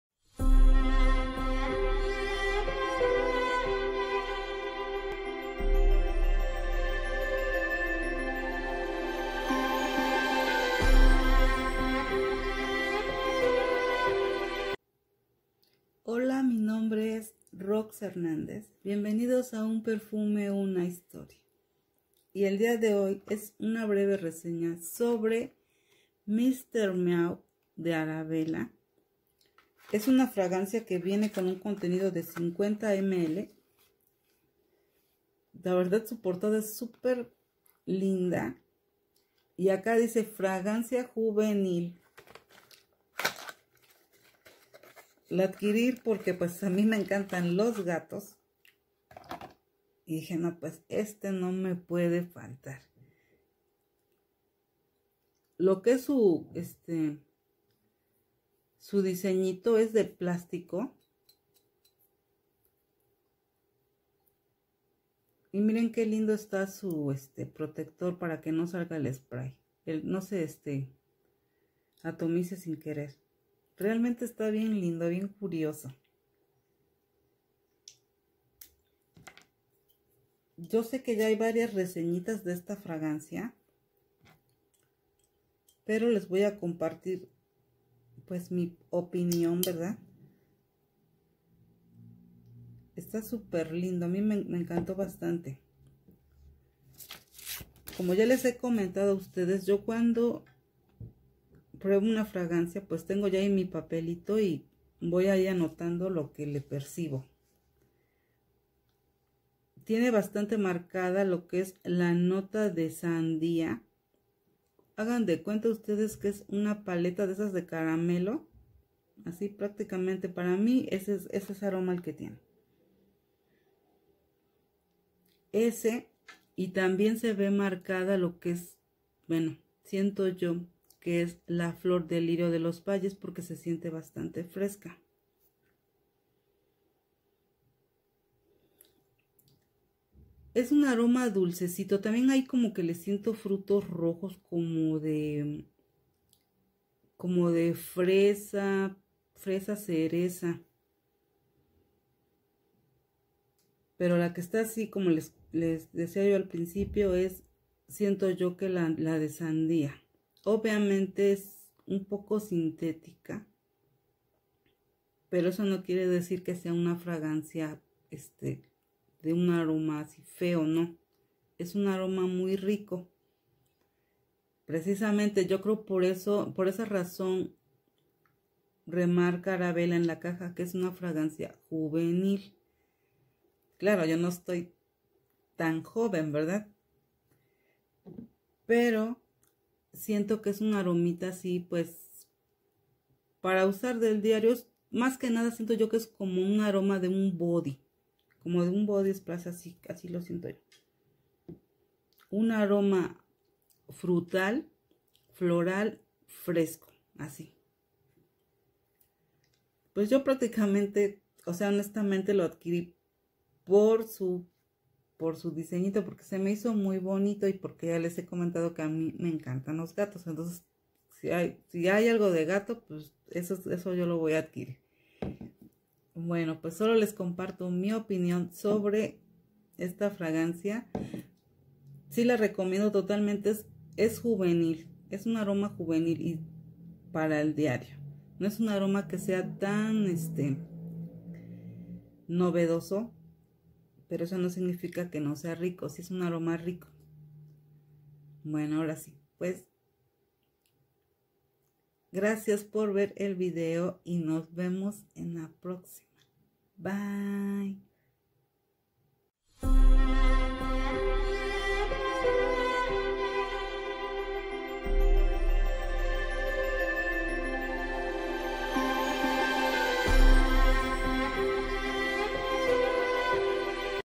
MR. MEAW DE ARABELA 💋
ESTA RESEÑA ESTA BASADA EN MI EXPERIENCIA CON ESTA FRAGANCIA

RECUERDEN CADA NARIZ ES UN MUNDO!